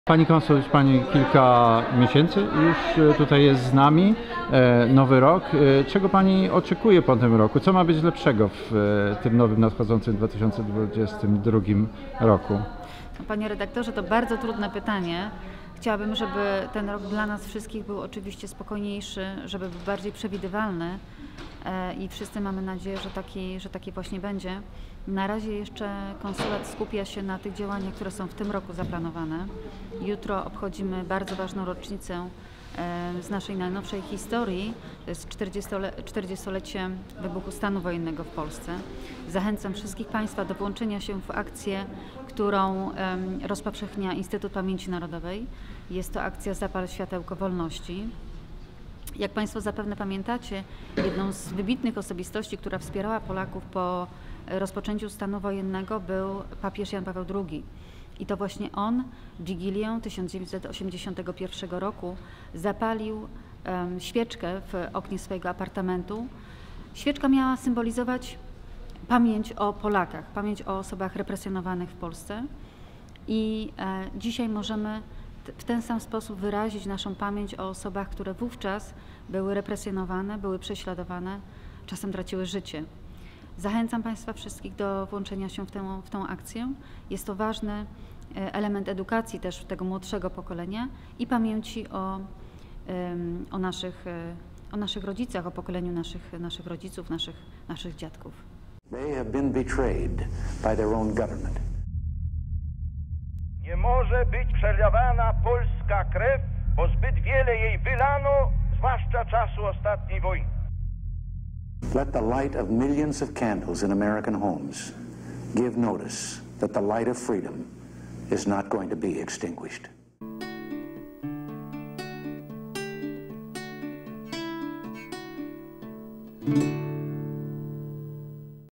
Zapal świecę w 40 rocznicę stanu wojennego
Mija 40 lat od wprowadzenia stanu wojennego, wydarzenia, które w dużej mierze ukształtowało historię współczesnej Polski...